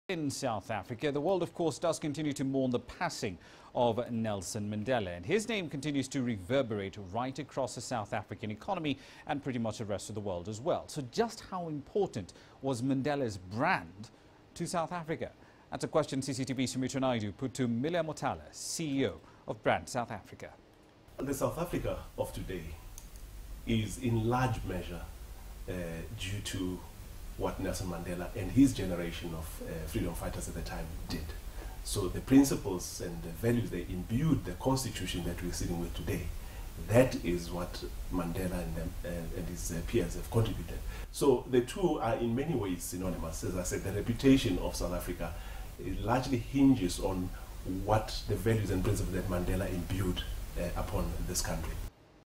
Brand South Africa to focus on continuing with Madiba's ideals
As the world continues to mourn the passing of freedom icon Nelson Mandela, his name continues to reverberate across the South African economy and the rest of the world. So just how important was the brand name Mandela to South Africa? That is the question CCTV'S Sumitra Nydoo put to Miller Motala, CEO of Brand South Africa.